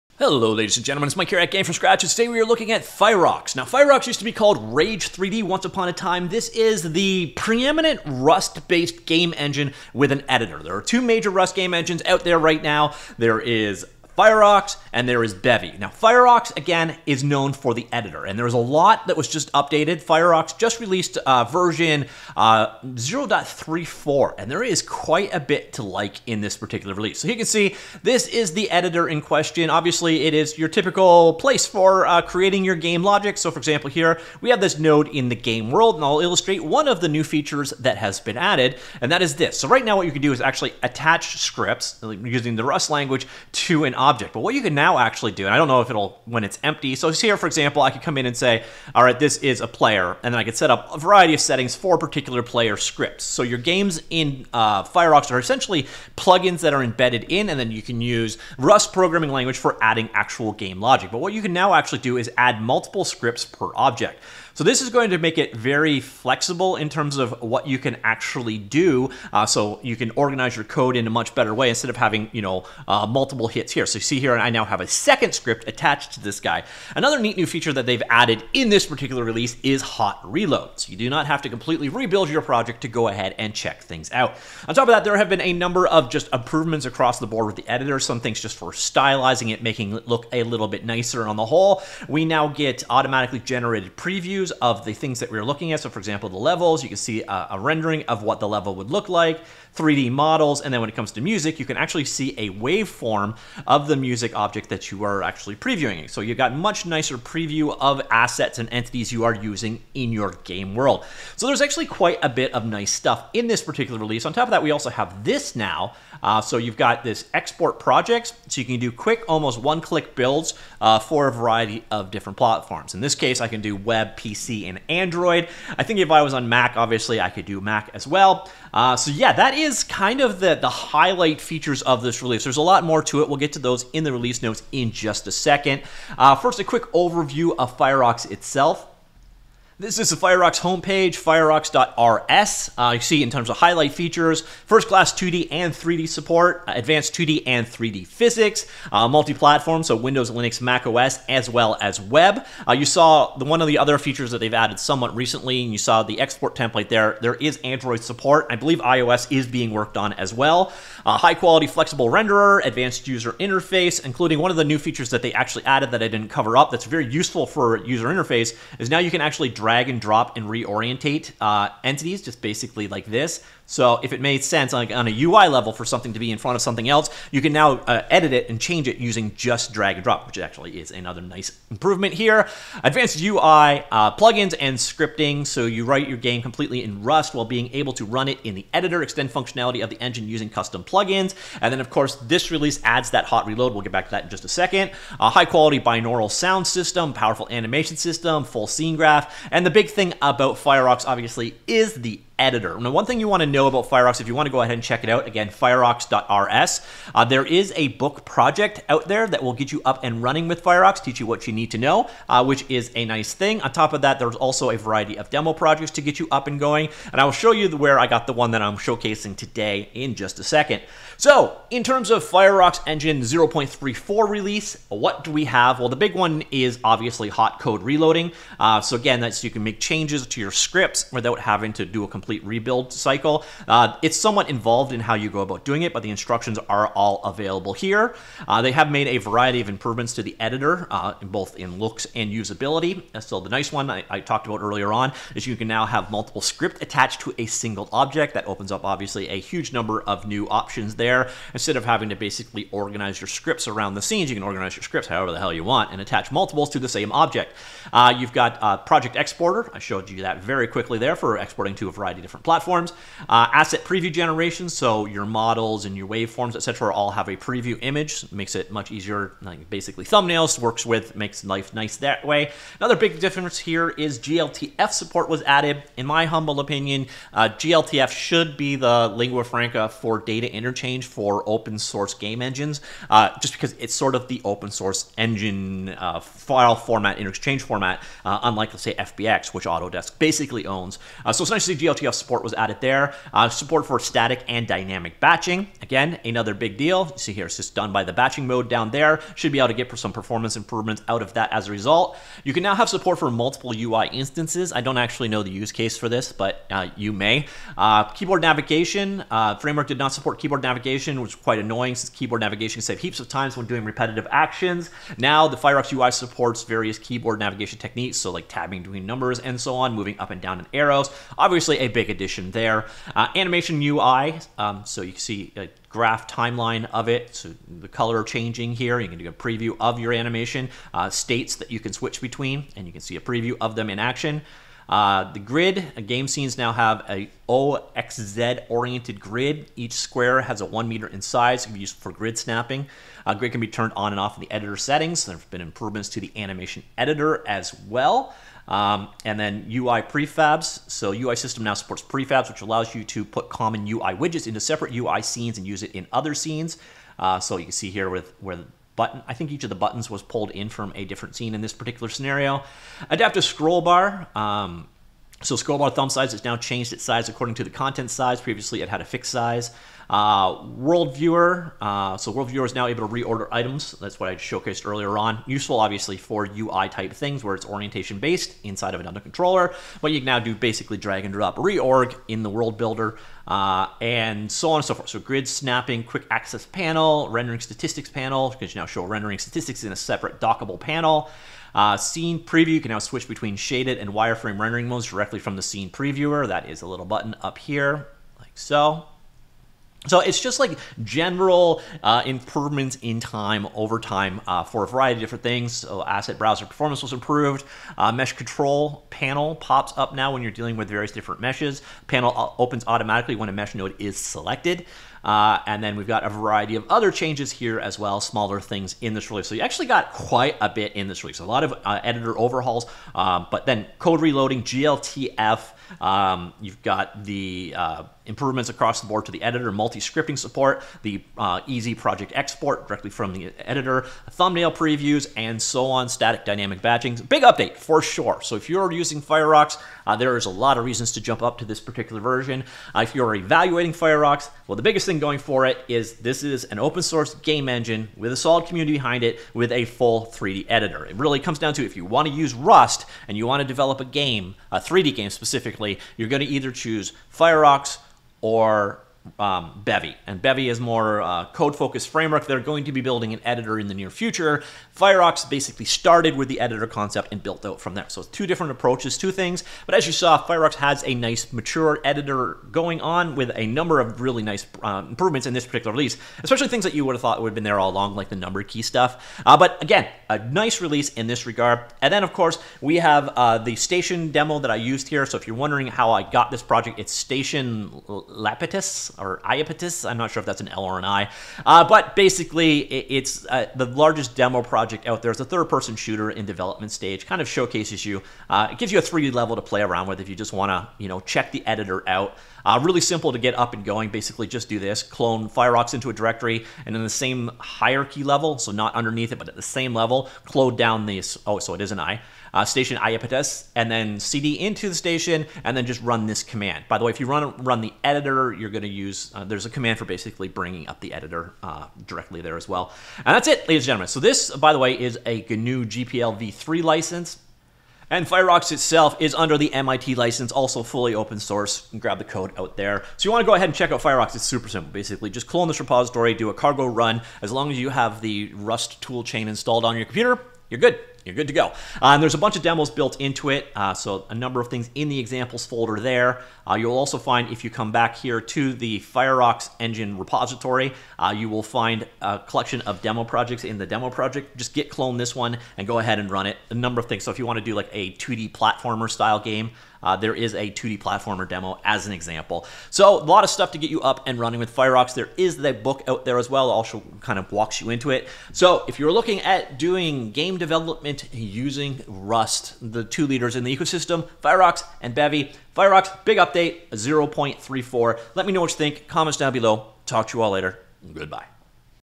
The Rust Powered Fyrox Game Engine Just Got a HUGE Update
The Rust based open source game engine Fyrox just released version 0.34 and this release has a ton of HUGE updates in it.  If you are looking for a Rust powered 2D/3D game engine with an editor, Fyrox is probably your choice.

In the 0.34 update we have things like
- hot reload
- multiple scripts per object
- project exporter
- asset previews
- much more

So in this video we take a look at the Fyrox game engine generally and at what's new in the Fyrox 0.34 release specifically.